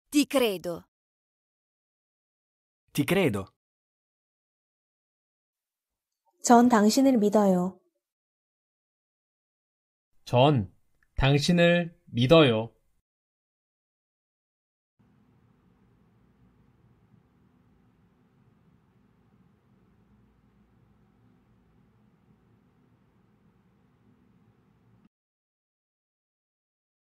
이탈리아어로 "전 당신을 믿어요." 말하는 법
남자 목소리 :
Ti credo.

여성 목소리 :
Ti credo.

남성 음성 번역 :
전 당신을 믿어요.

여성 음성 번역 :
전 당신을 믿어요.